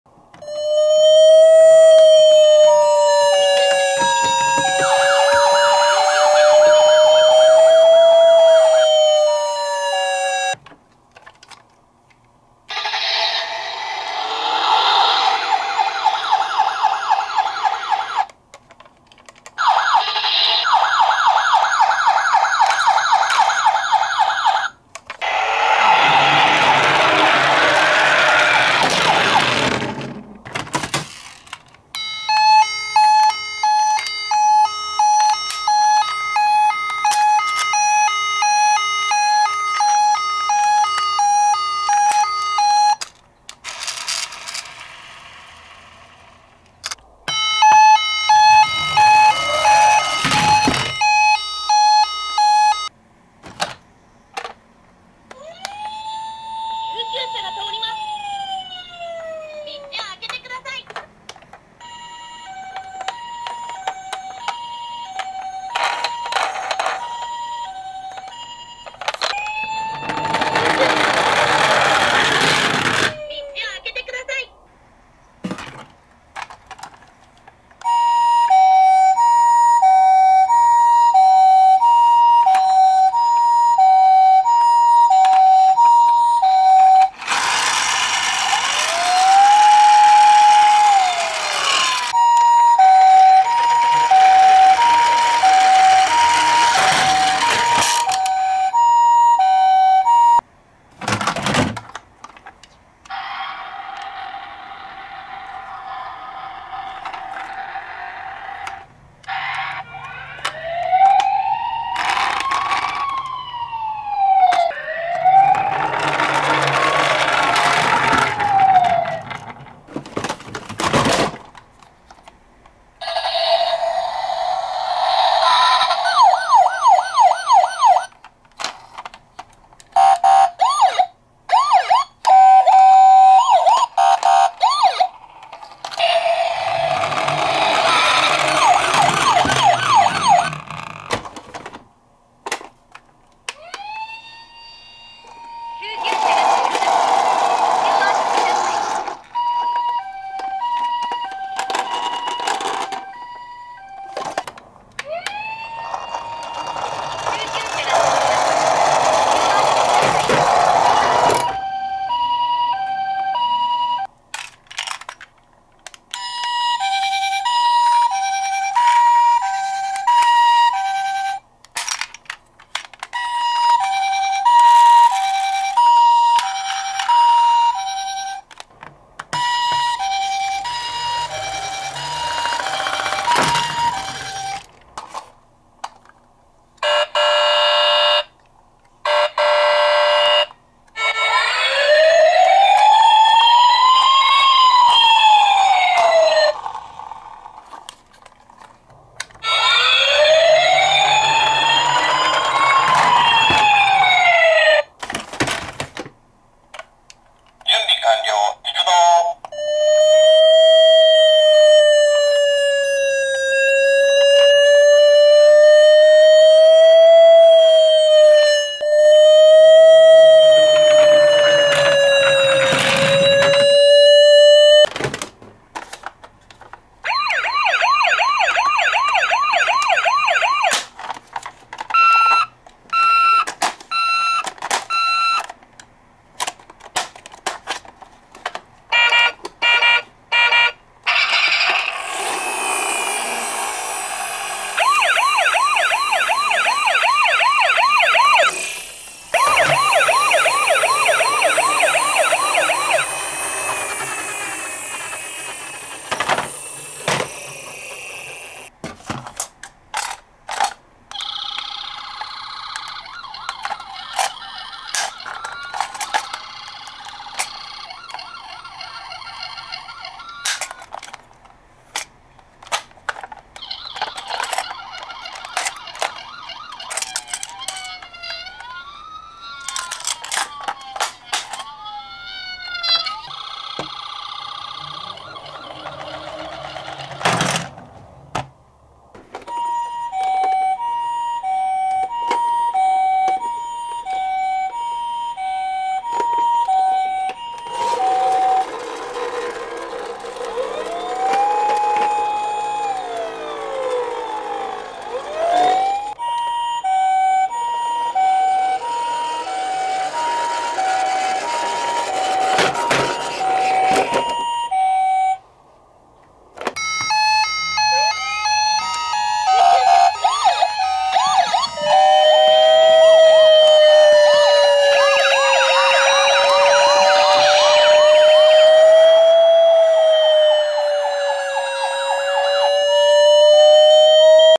箱から救急車取り出して細かくチェック 坂道をサイレンならして走るよ
最新動画